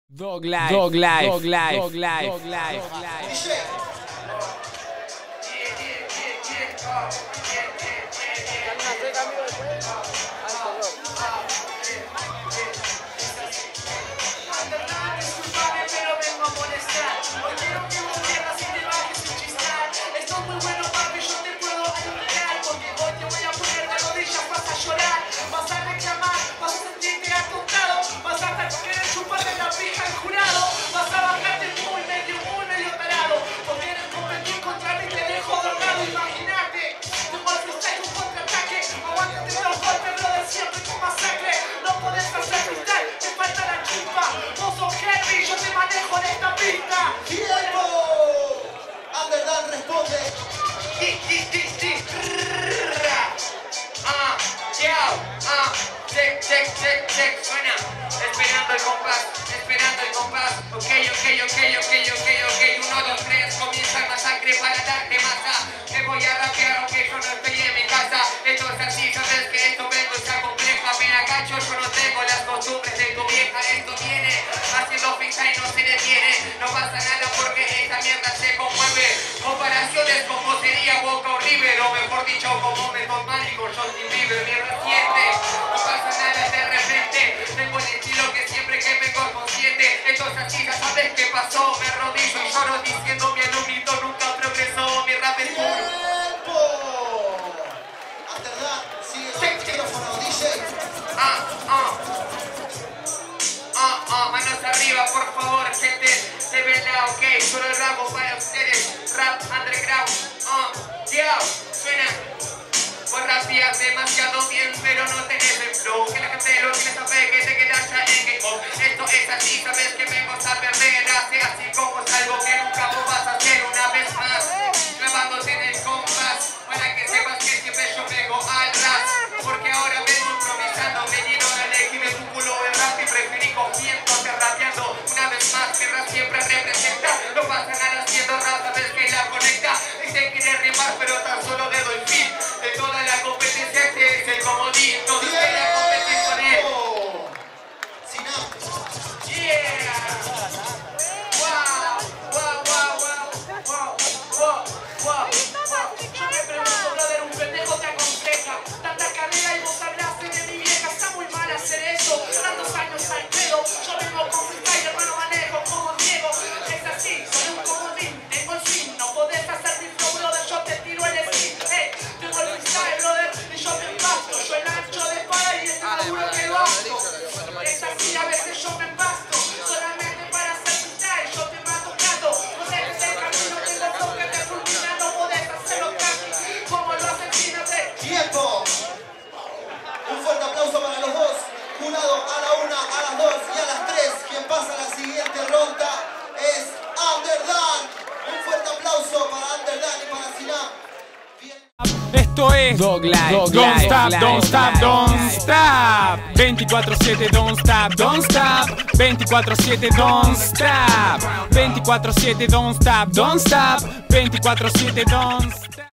Underdann vs Sinaph - 1ra ronda Habla Hispana Vol 2
|| Gana Underdann contra Sinaph en la primera ronda de la competencia de Freestyle del Habla Hispana Vol. 2 celebrada el domingo 17 de Noviembre de 2013 en la Sociedad de Fomento de El Jaguel.